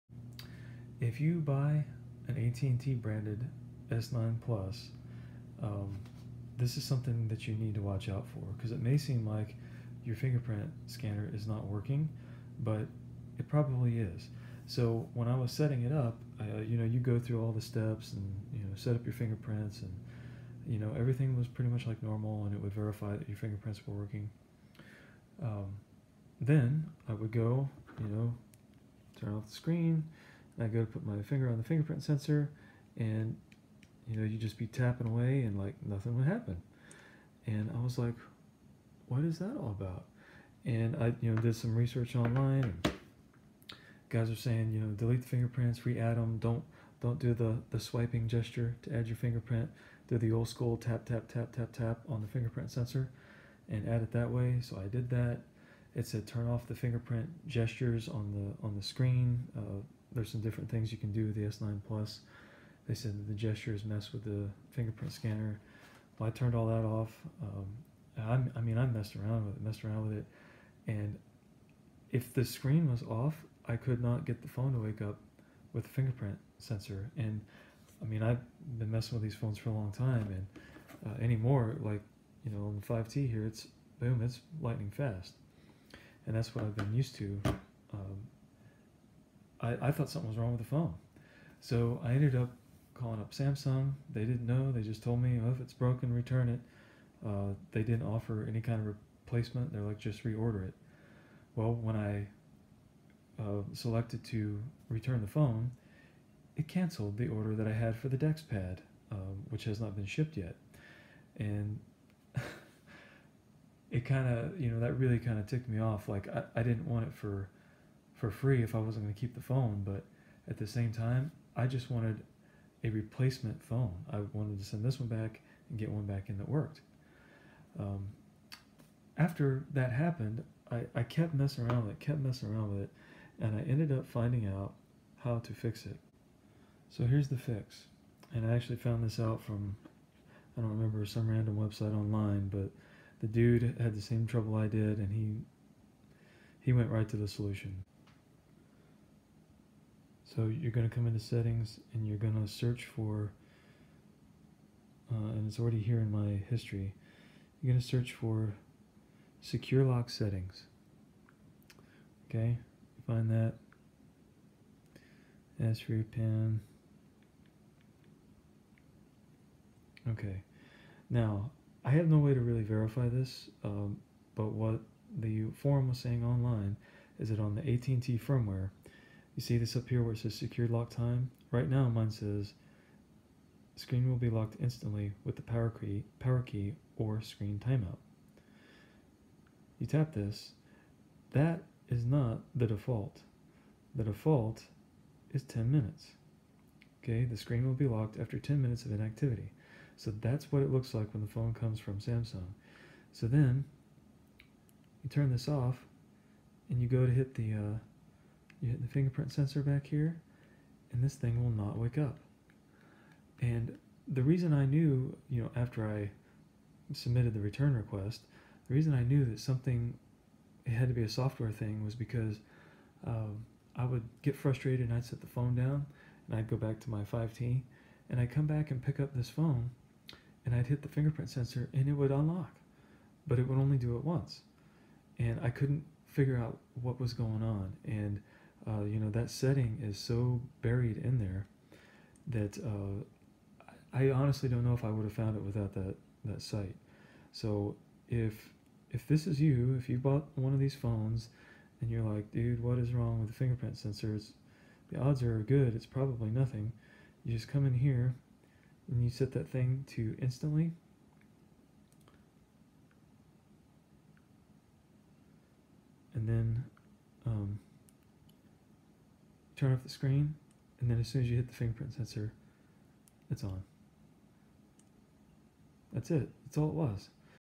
Fix S9 S9+ Fingerprint Sensor Stops Working
This video will show you how to fix an S9 or S9+ fingerprint sensor that doesn’t seem to be working, or only works once. It’s a setting in the software when you have an AT&T branded S9.